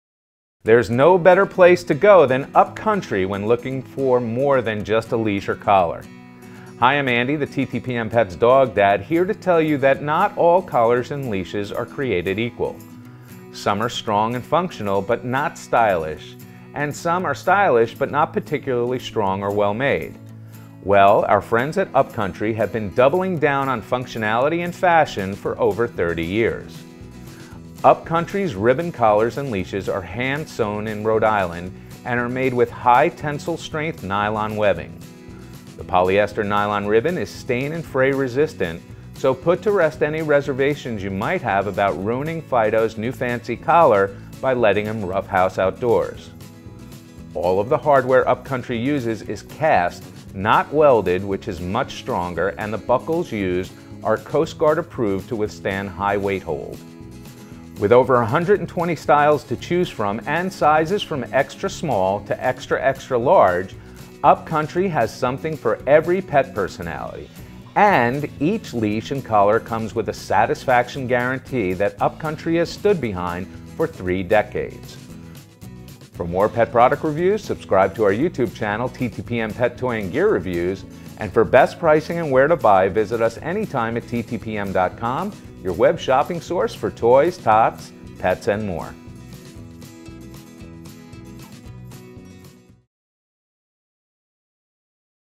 Plaid Dog Lead & Plaid Dog Collar, Medium from Up Country
Up Country Dog Collars and Leashes. High Quality Designer Patterns with Affordable Prices. For full review and shopping info

Product Info: Not all collars and leashes are created equal.  Some are strong and functional but not stylish and some are stylish but not particularly strong or well made and you usually get what you pay for.  Up Country has been doubling down on functionality and fashion for over thirty years with prices that are affordable to most pet parents.  Up Country�s ribbon collars and leashes are hand-sewn in Rhode Island and are made with high-tensile strength nylon webbing.  The polyester nylon ribbon used is stain and fray resistant so put to rest any reservations you might have ruining Fido�s fancy new collar or leash by letting him roughhouse outdoors.  All of the hardware Up Country uses on it's leashes are solid cast brass, not welded, which is much stronger.  Also, the buckles used on the collars are Coast Guard approved to withstand high weight hold. With over 120 styles to choose from  Up Country has something for every pet personality. Each leash and collar comes with a satisfaction guarantee that Up Country has been standing behind for 3 decades.  The Up Country leashes and collars are machine washable.